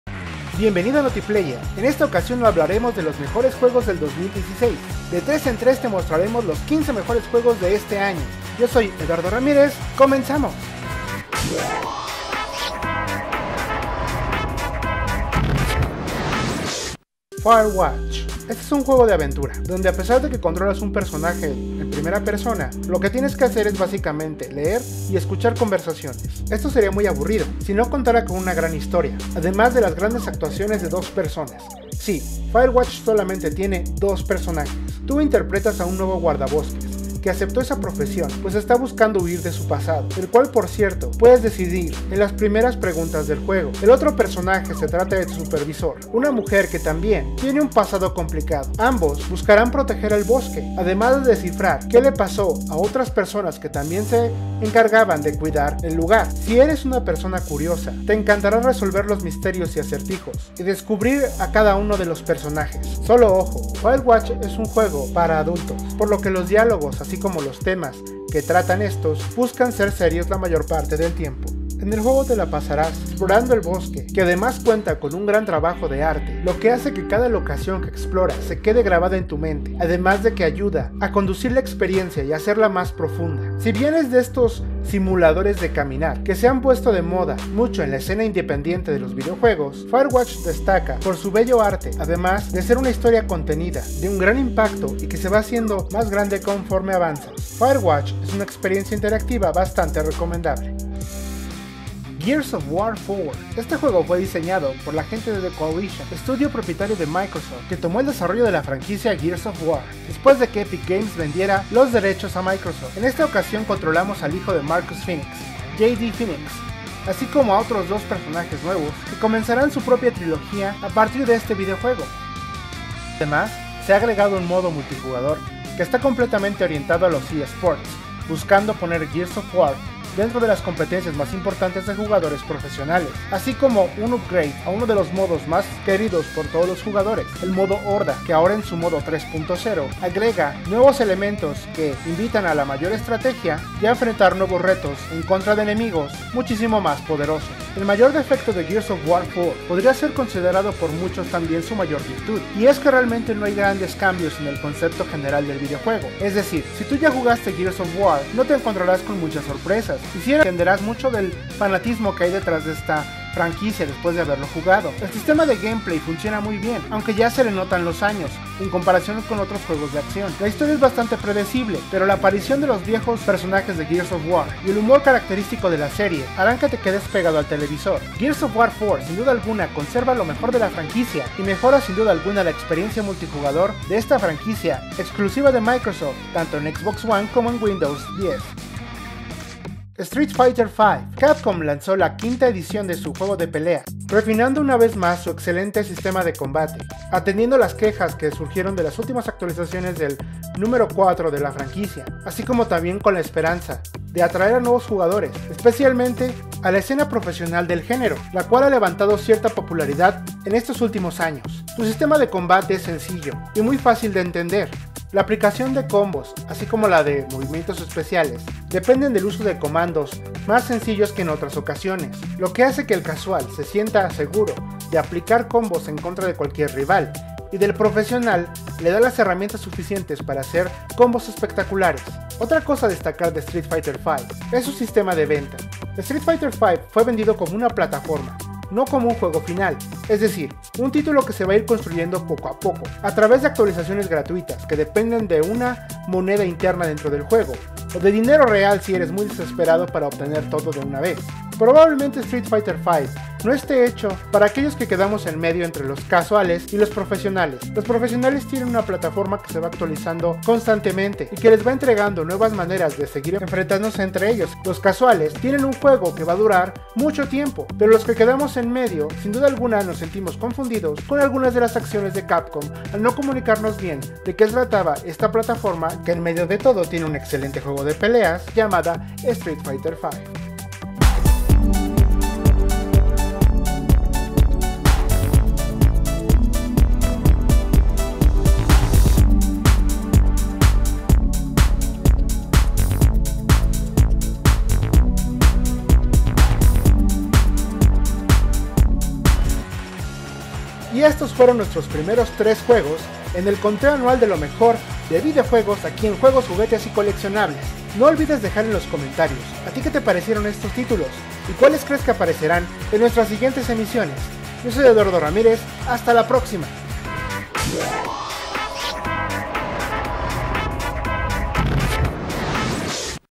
Los mejores videojuegos del 2016 #1 ★JJyC★
Los mejores videojuegos del 2016 Vol. 1 - De tres en tres y de aquí al 2017 te presentaremos los 15 títulos que hemos elegido aquellos que marcaron el año y algunas menciones honorificas que vale la pena considerar. Deja en los comentarios tus favoritos o quienes crees que estarán en nuestro conteo.